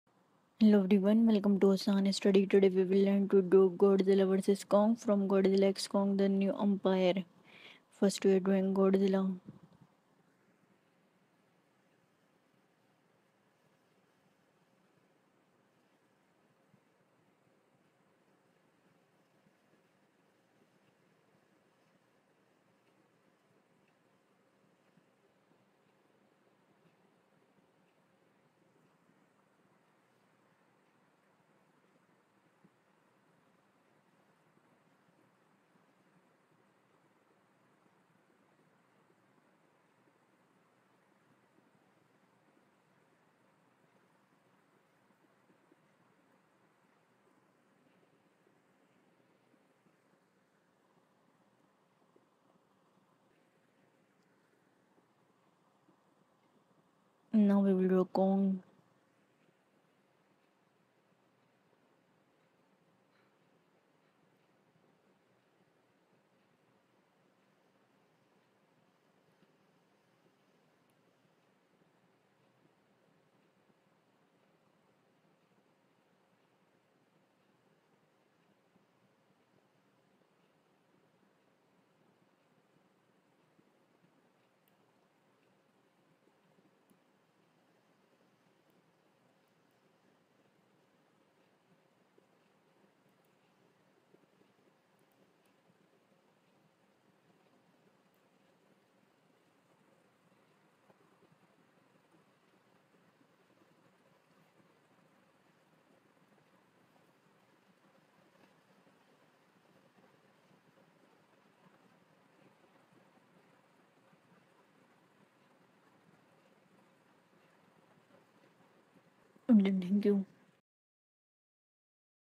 How to draw Godzilla vs Kong From Godzilla x Kong The New Empire | Gozdilla vs Kong Running
In this video we are going to draw godzilla vs kong running from Godzilla x Kong The New Empire in an easy way...!